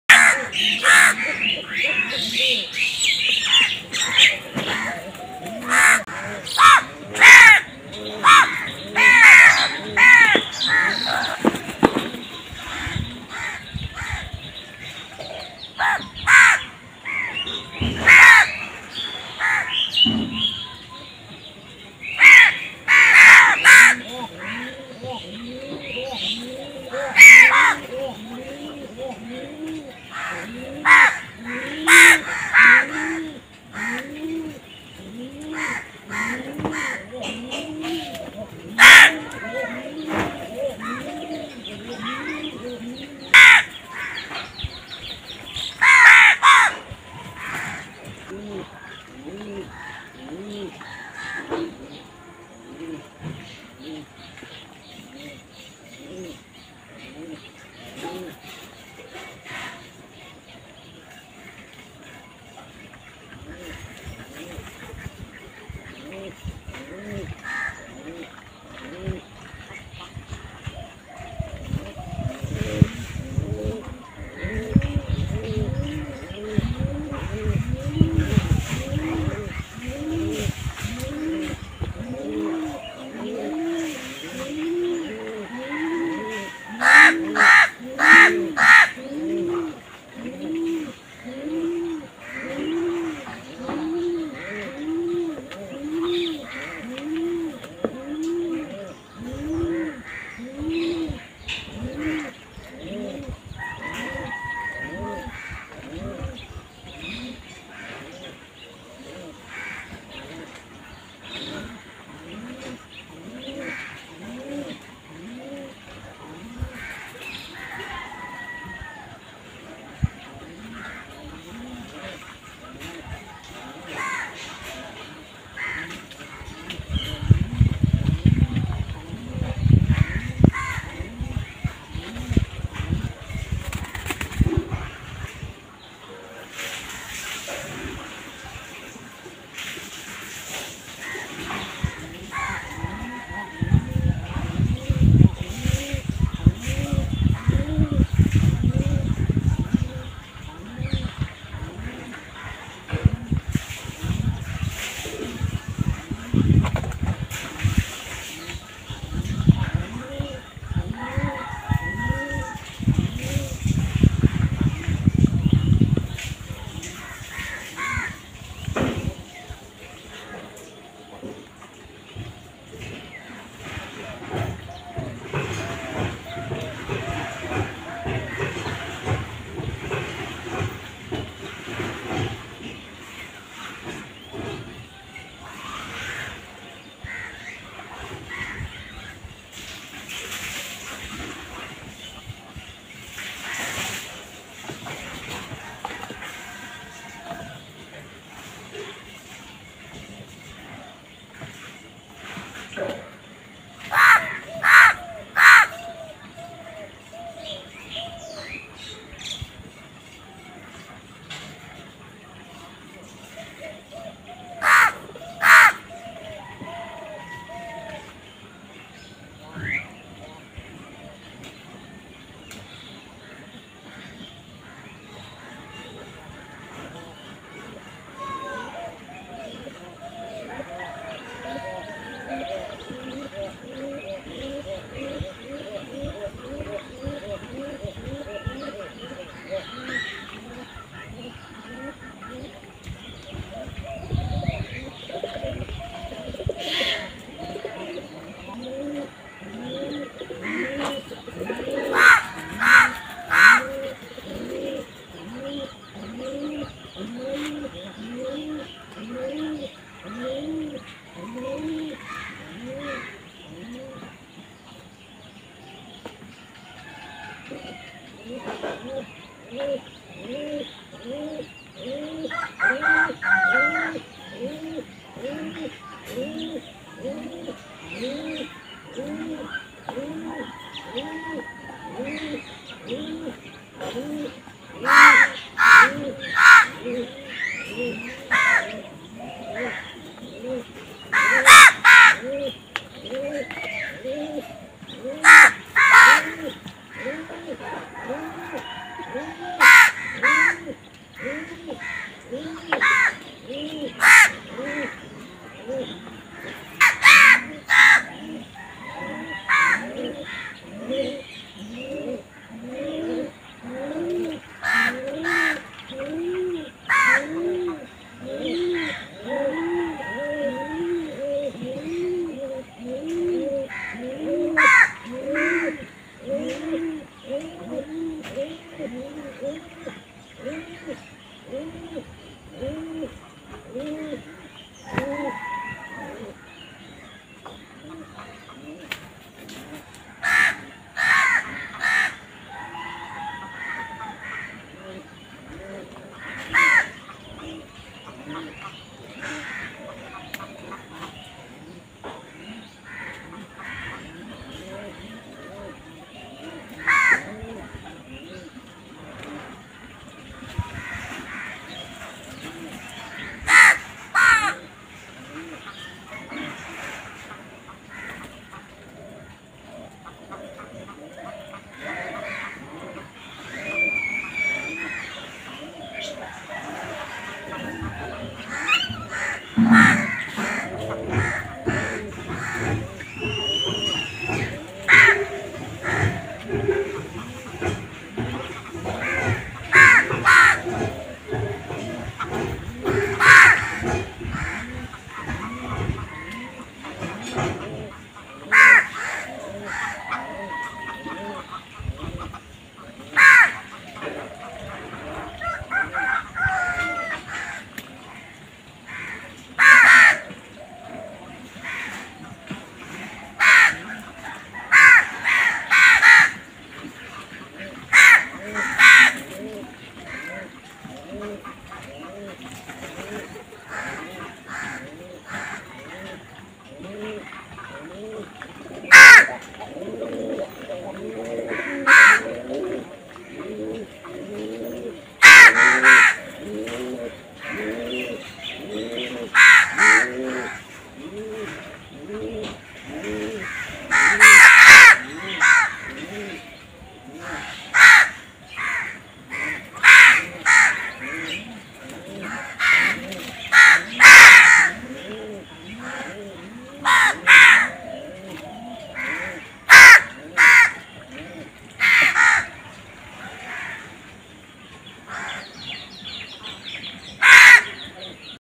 Funny crow compilation Video 2021 | Crow Bird Crowing Sound Part 46 | Bird and animal sound
So, Crow Sound part 46... Well, the video gives you more fun and you can find how to crow fight for food
Hey guys! hope you like this Crow Sound Video